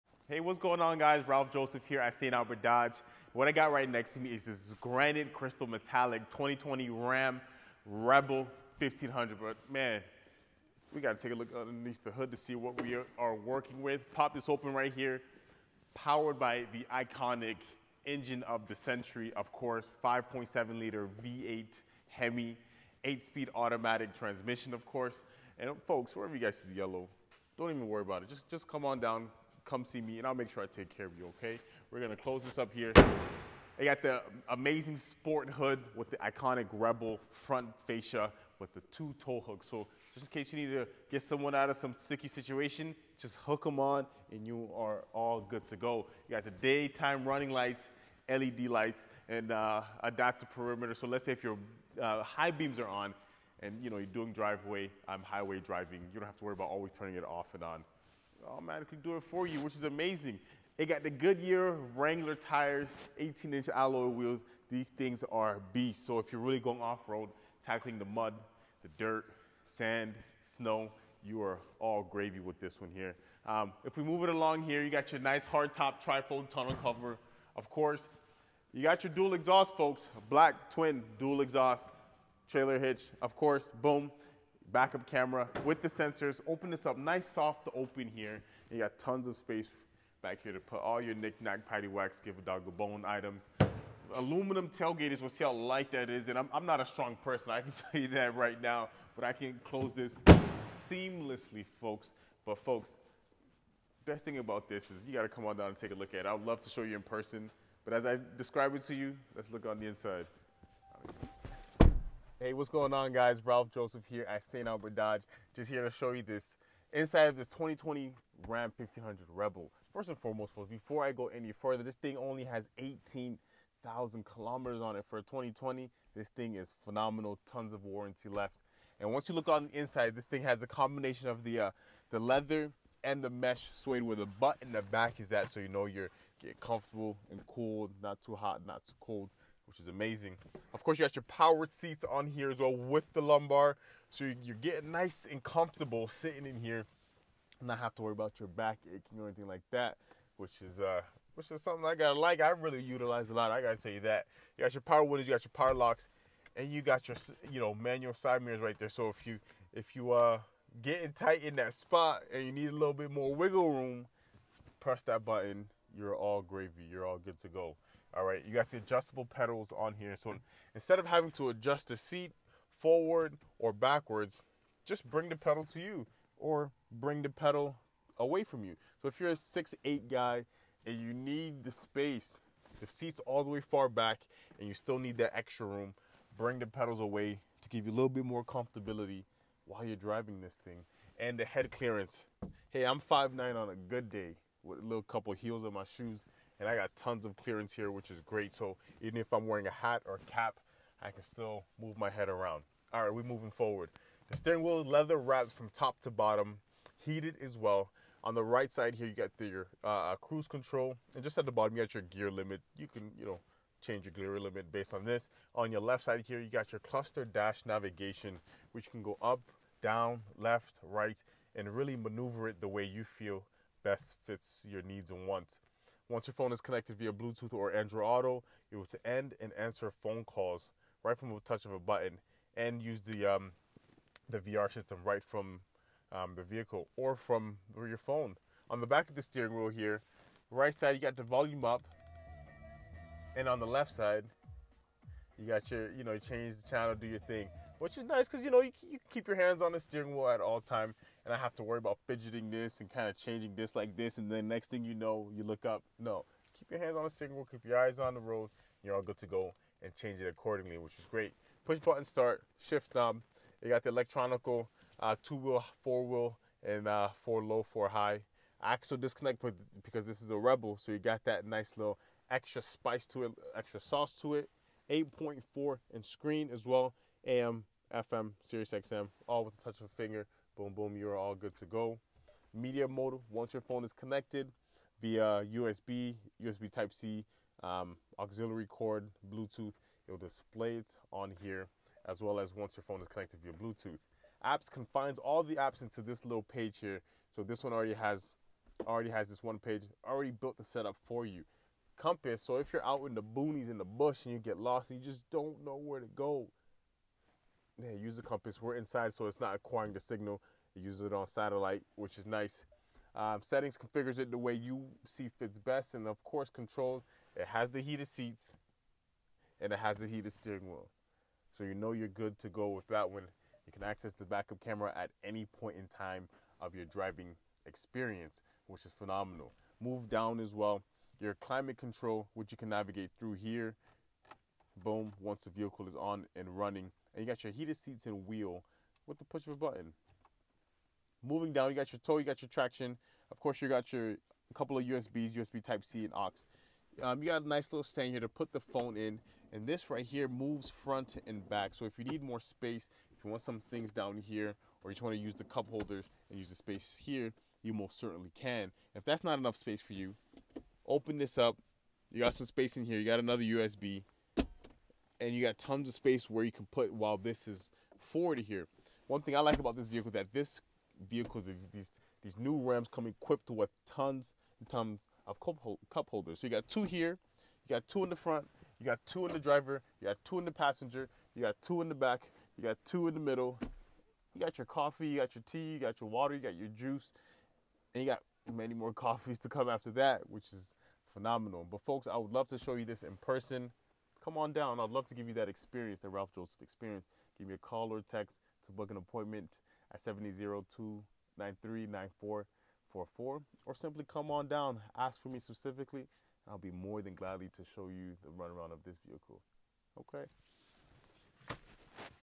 2020 RAM 1500 Rebel | Stock 1GH8312A | St. Albert Dodge
Check out this 2020 RAM 1500 Rebel | Only 18,143 km - Heated Seats - Heated Steering Wheel - Apple Carplay - 8.4" Touchscreen - One Owner

Stock: 1GH8312A
VIN: 1C6SRFLT0LN419357

1155 St Albert Trail
St. Albert, AB T8N 7X1
Canada